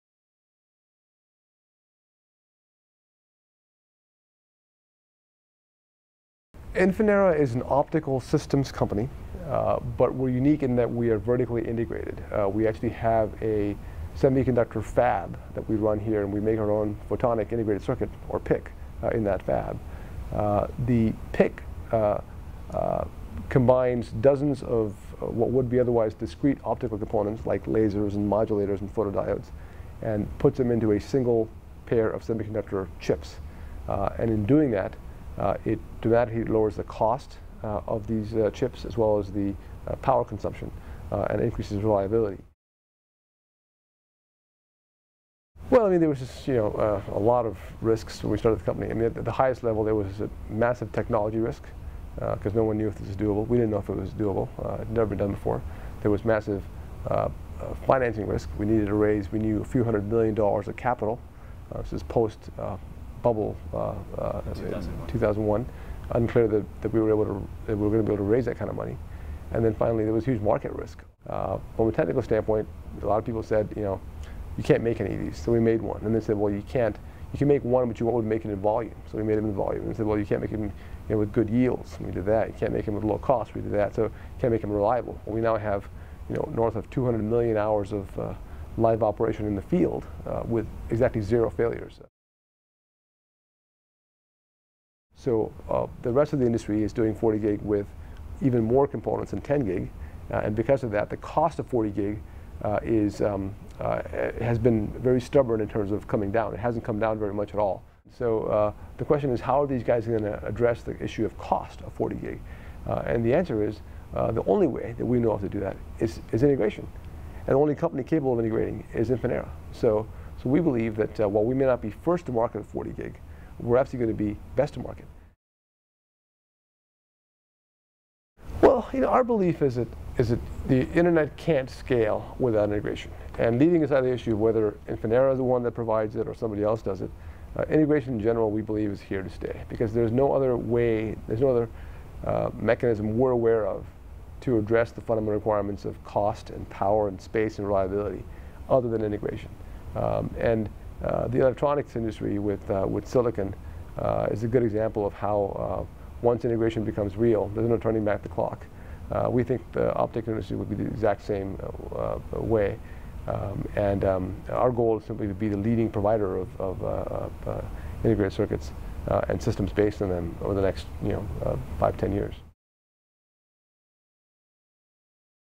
Interview with former CEO of Infinera Jagdeep Singh
Video Interview for wsj.com of Infinera Chairman of the Board and former-CEO Jagdeep Singh. Produced and Edited by Jesse Lu and Drake Martinet. Collaboration with wsj.com centered around the theme "Innovation".

Infinera makes optical communication equipment that powers the Internet. Their main technological innovation is the Photonic Integrated Circuit (PIC), which crams dozens of lasers and photodiodes onto a single chip.